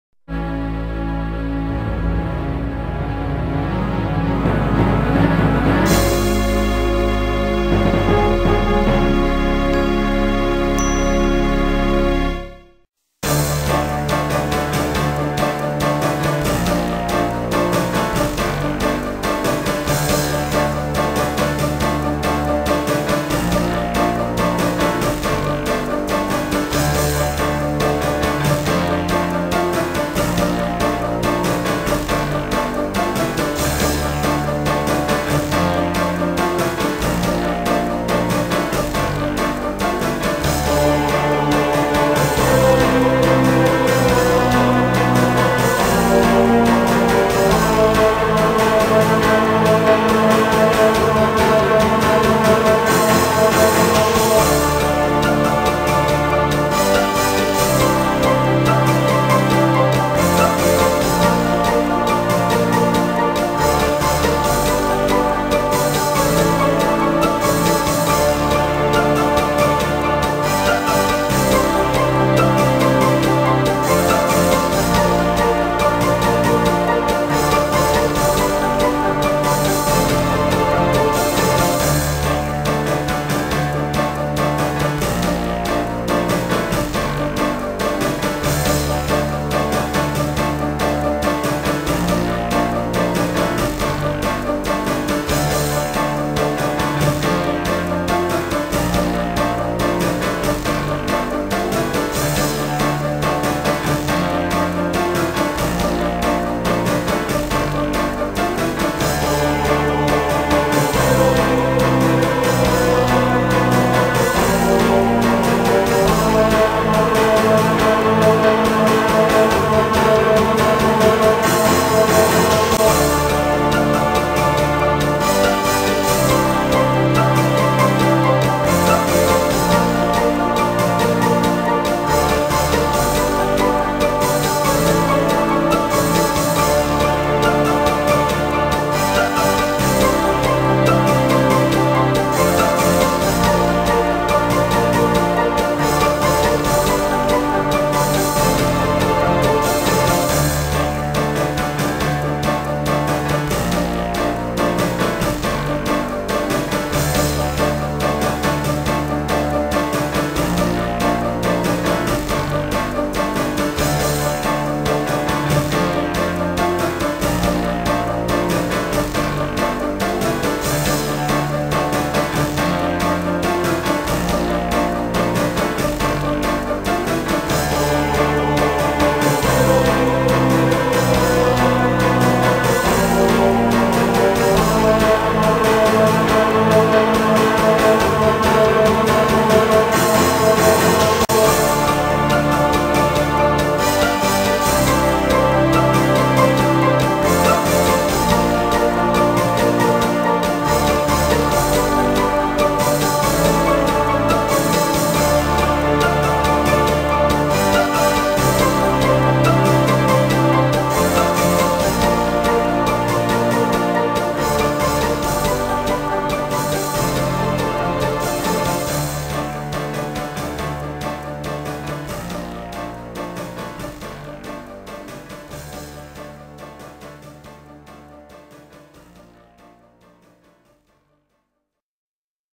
Sierra Chest 1000th video: a compilation
Just a few days short of the 4th anniversary of the website and with the 1000th subscriber join up today, I made the 1000th video upload a special one: a compilation of 4 years of work in one video! Can you guess all the games?